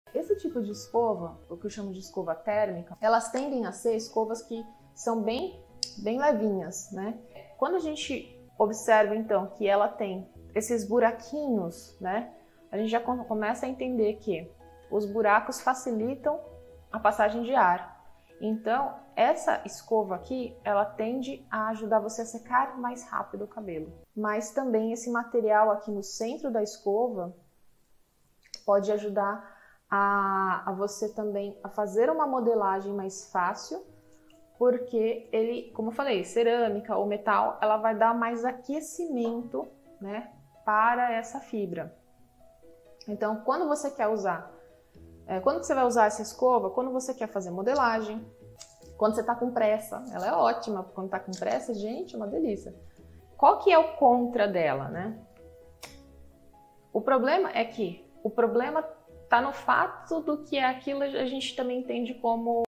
Quando usar escovas térmicas
Tem dúvidas sobre as escovas térmicas?

Se você está com pressa, elas são excelentes para modelar mais rápido, porque o tipo de material dá mais aquecimento para a fibra.

Esse tipo escova também não adere tão bem no cabelo, por isso que cabelos que são mais difíceis de alisar, você vai ter mais dificuldade.

📲Entre para o nosso Canal do Telegram e receba todas as novidades